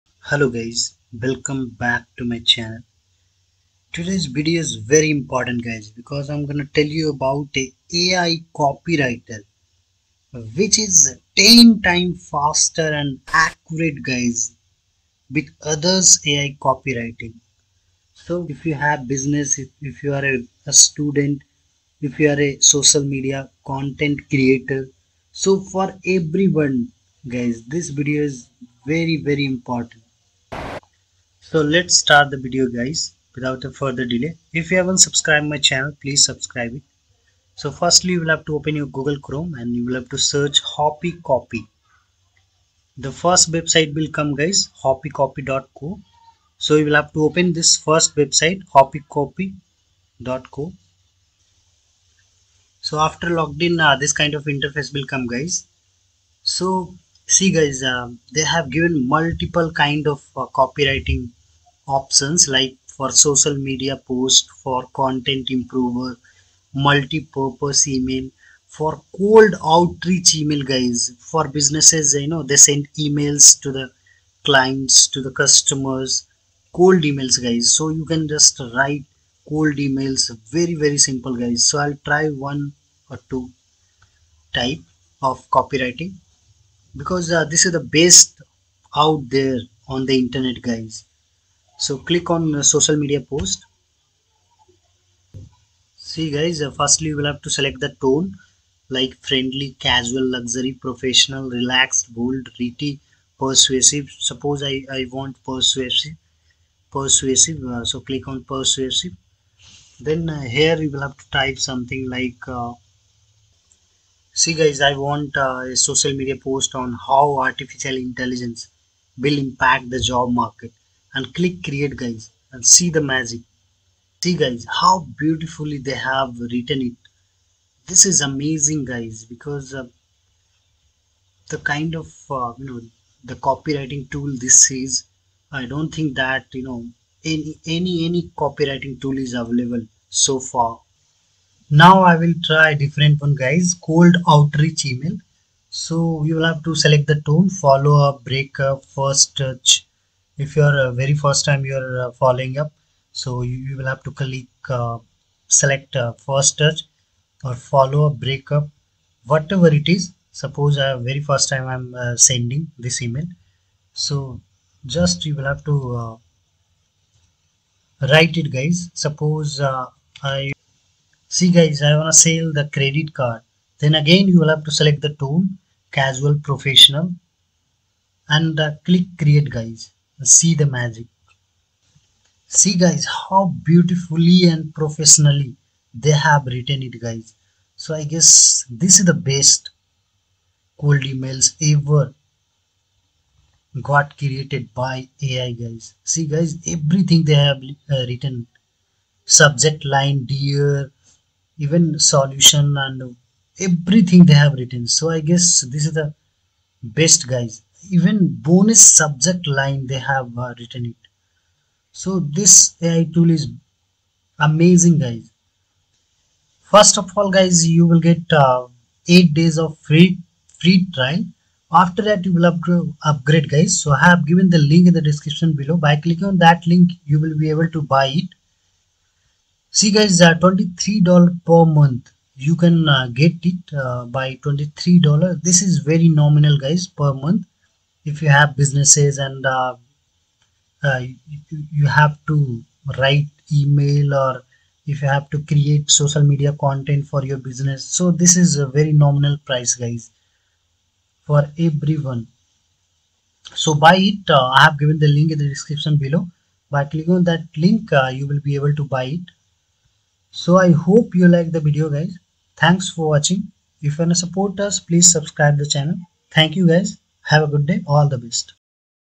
World Best AI Copywriter | Hoppy Coppy AI Copywriter Tutorial
In this video you will get to know about the one of the best copywriter tool hoppy coppy. This is world best copywriting tool and very convenient to use. If you waana buy their upgrade version i have given the link down , by click on this link you will be able to upgrade your hoppy coppy ai tool.